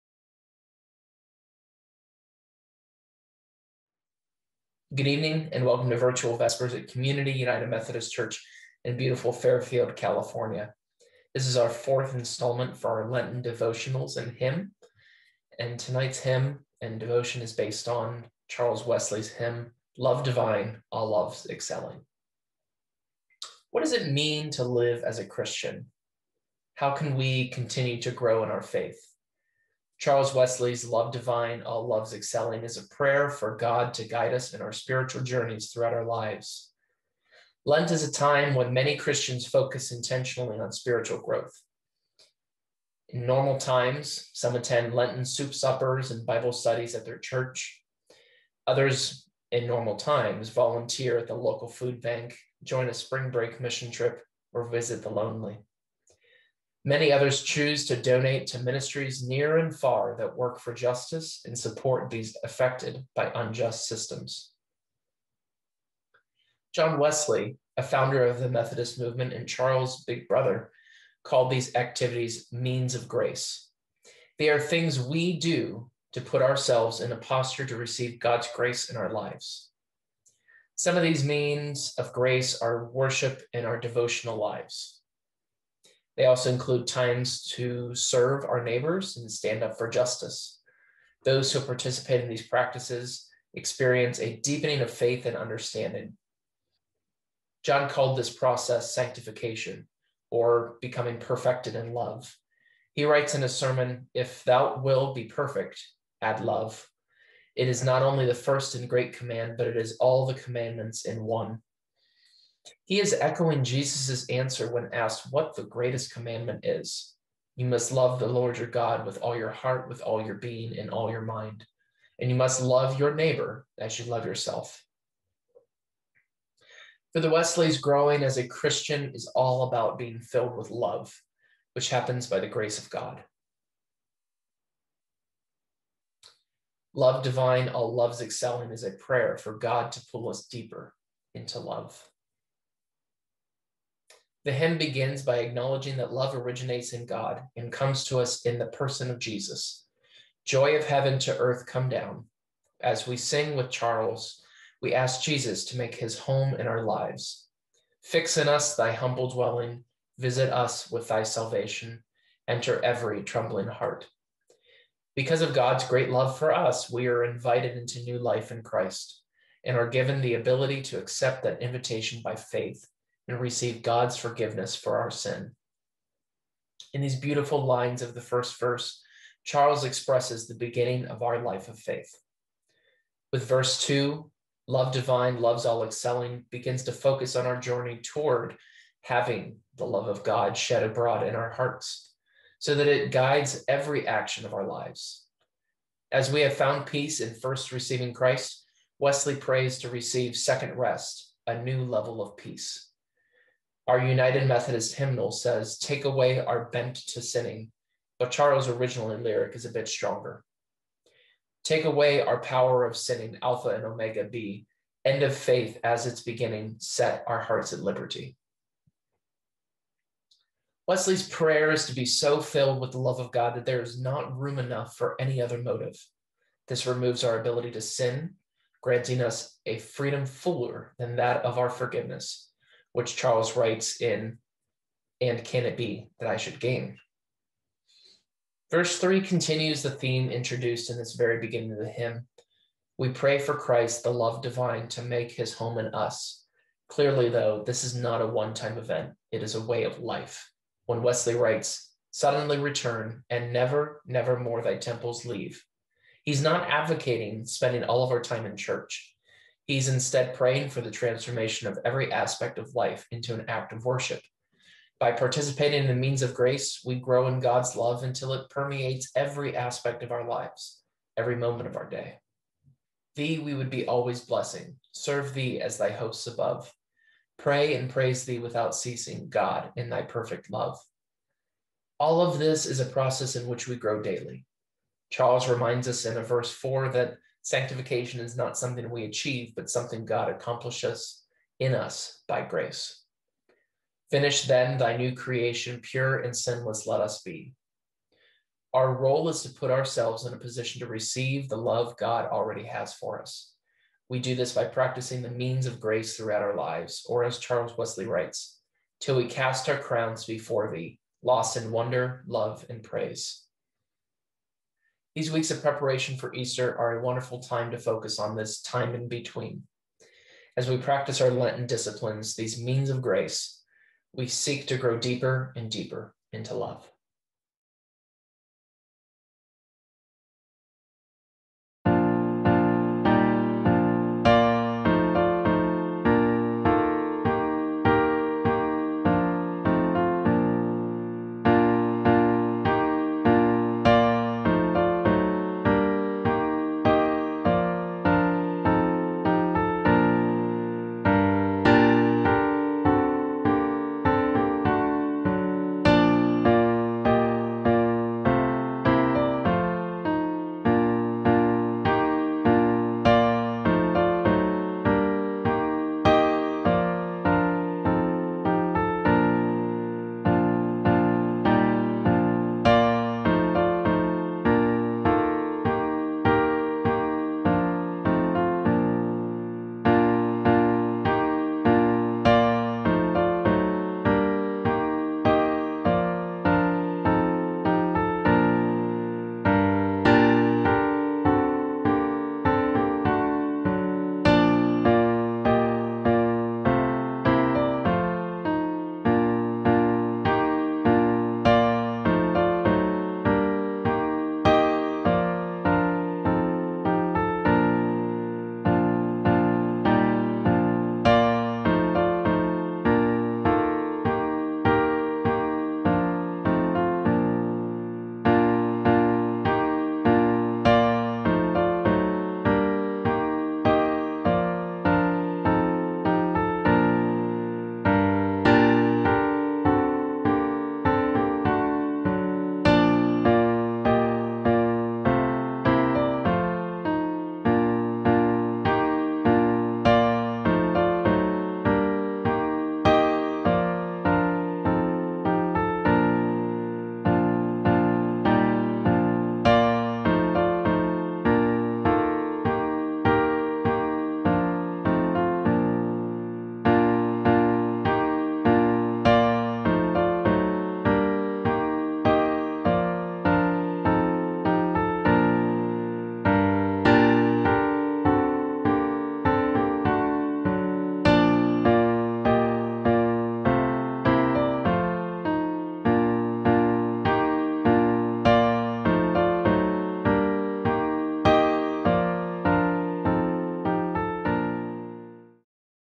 Community UMC: Virtual Vespers for Lent - Week 4
Welcome to Community UMC's Virtual Vespers. In this current state of physical distancing and turmoil around us, our hope is to provide a few moments of reflection, musical meditation, and prayer to help keep us connected to God and to each other during these trying days.